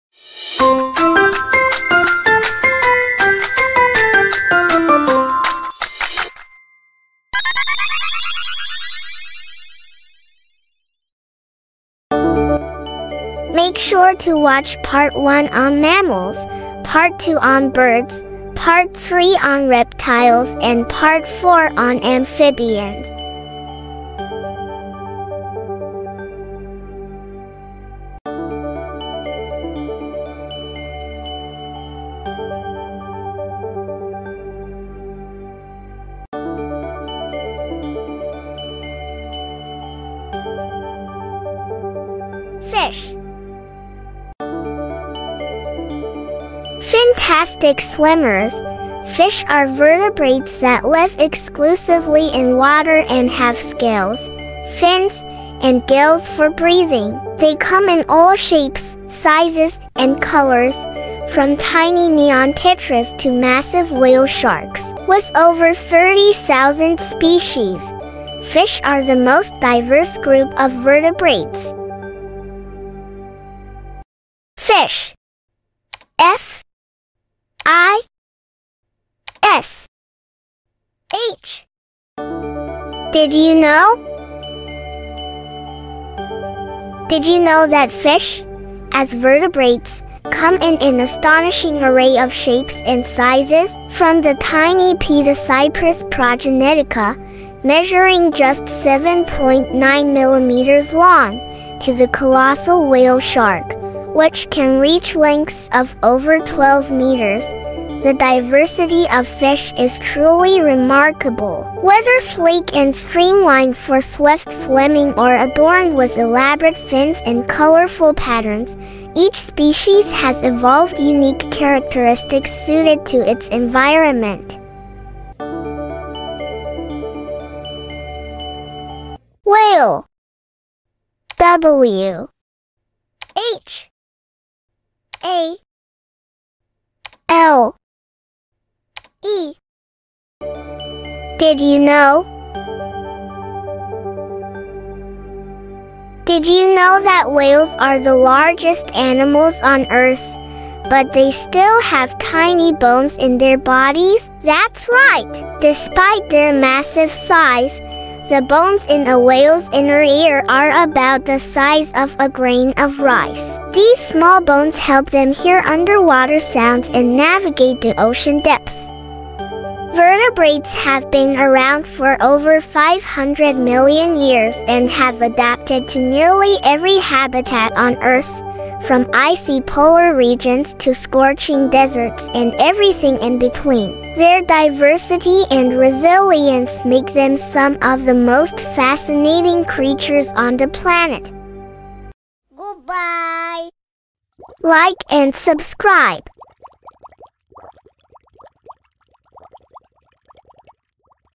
Unlocking the Wonders of Fish: Exploring Vertebrate Diversity!
Grab your snorkel because today we're exploring the ocean depths to uncover fascinating facts about fish! From colorful clownfish to graceful sharks, we'll discover the incredible diversity of vertebrates that call the sea their home.